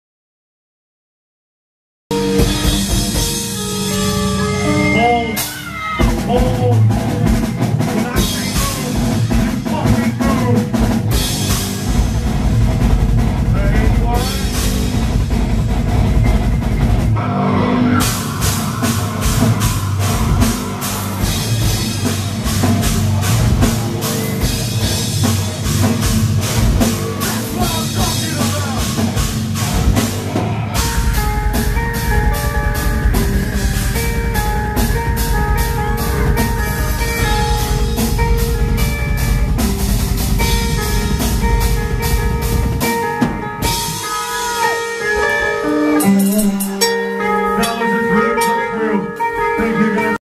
Wall Of Death @Jam Spot 12/3/2016
Alex Viola of Polyhedra instructed the crowd into a wall of death. Enjoy!!!